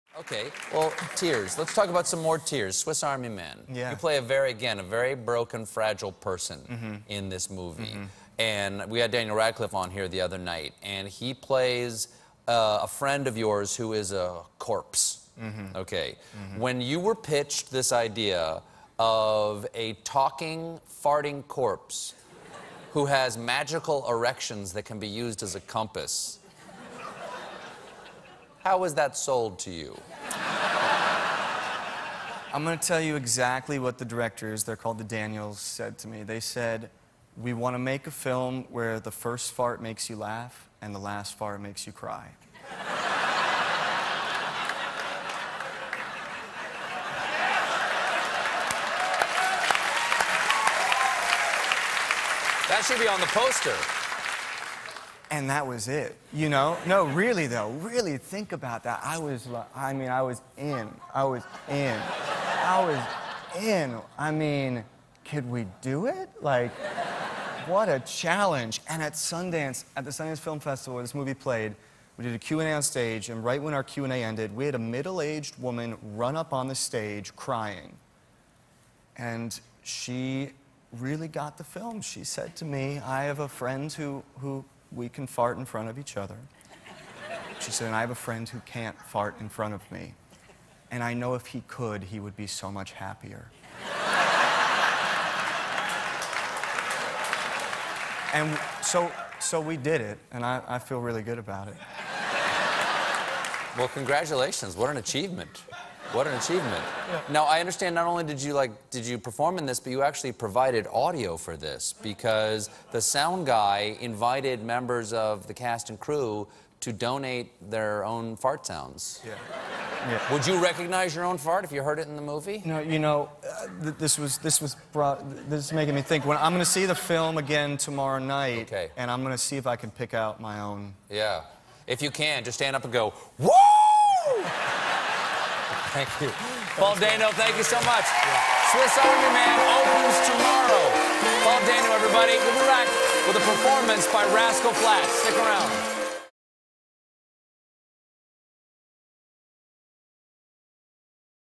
"The First Fart Makes You Laugh, The Last Fart Makes You Cry"
With those magical words, the directors of the movie "Swiss Army Man" convinced Paul Dano to sign on for the lead role.